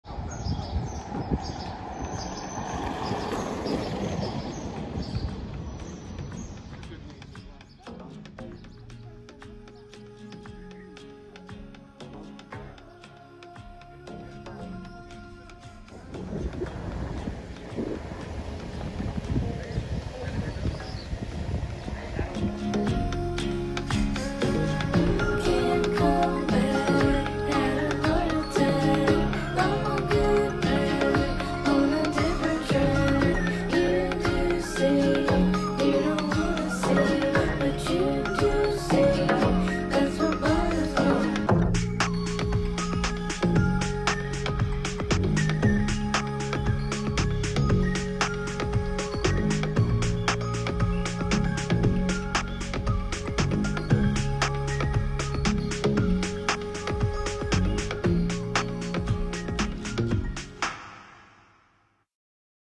Birding Day tour
Our socially distanced Birding day tour this week - highlights included Little Swift, Lesser Short-toed Lark, White-headed Duck, Little Bittern, Red-knobbed Coot, Spanish Imperial Eagle (2) and (3) Rufous-tailed Bush-robin